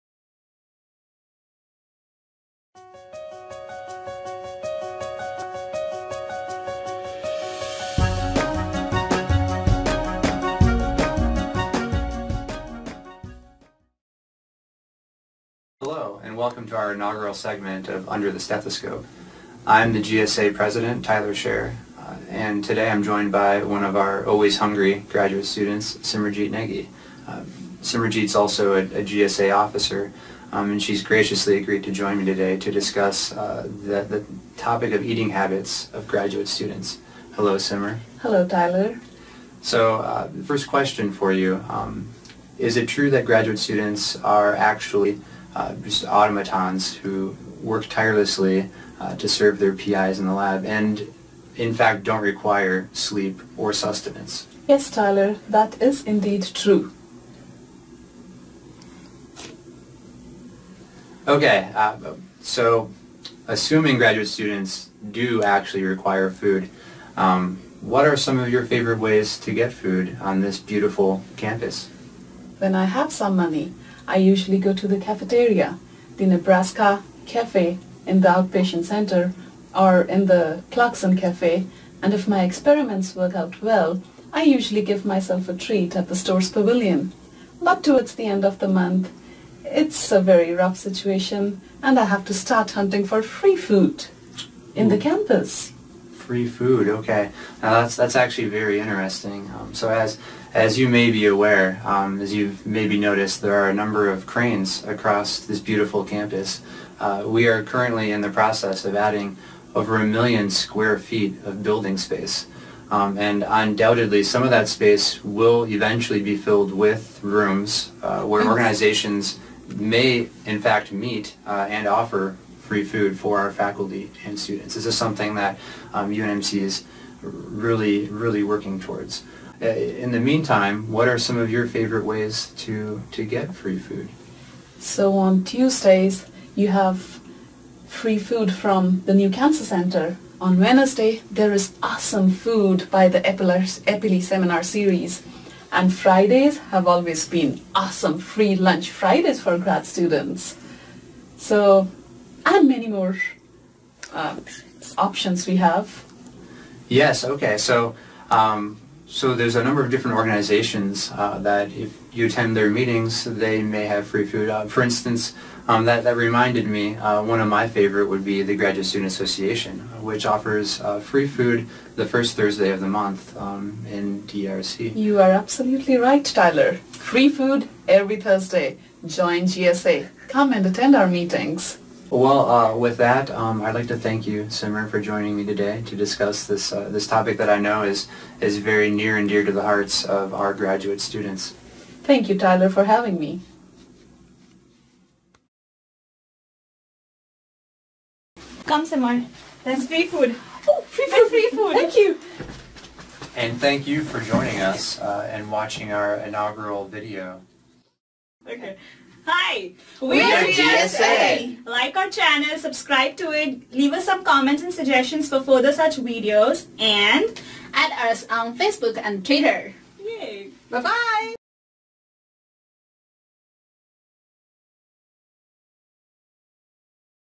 Under the Stethoscope with the UNMC GSA: Finding Free Food
Join the UNMC GSA for a discussion regarding the plentiful opportunities for free food that can be found on campus. For a list of UNMC seminars that offer food, check out the GSA Blog